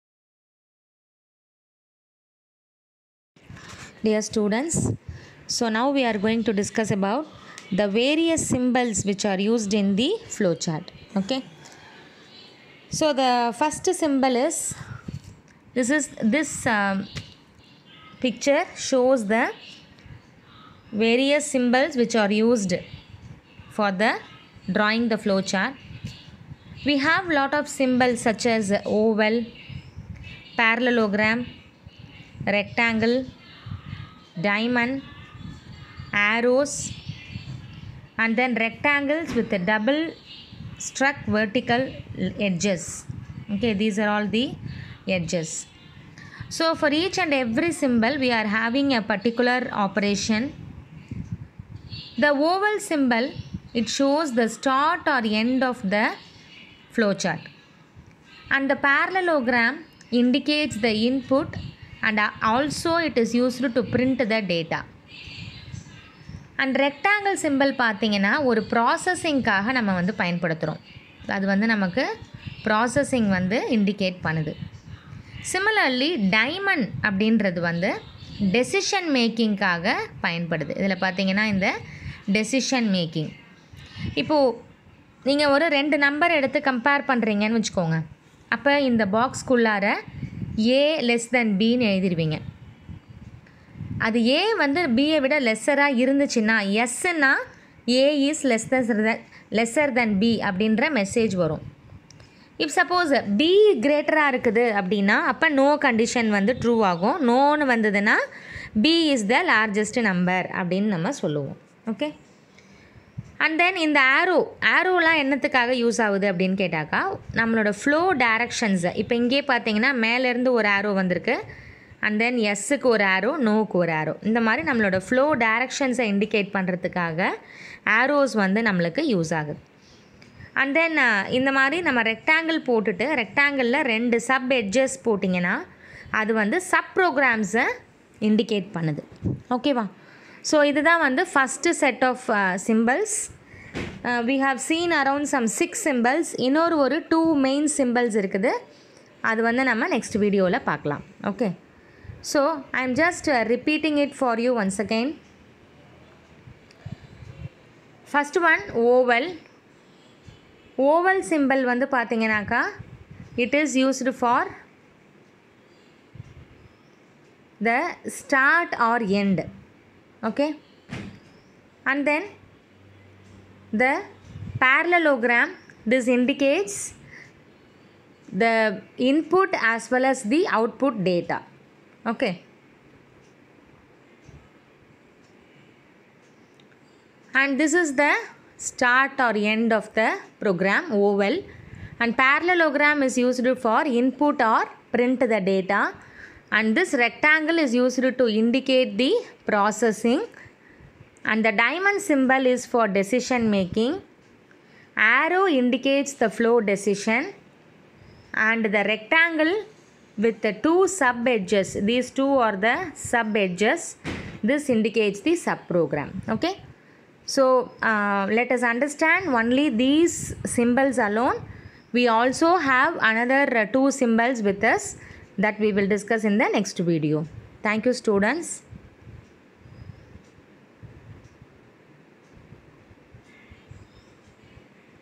FLOWCHART SYMBOLS PART 1
This video describes the Flowchart symbols. Covers the DOTE syllabus Programming in C language.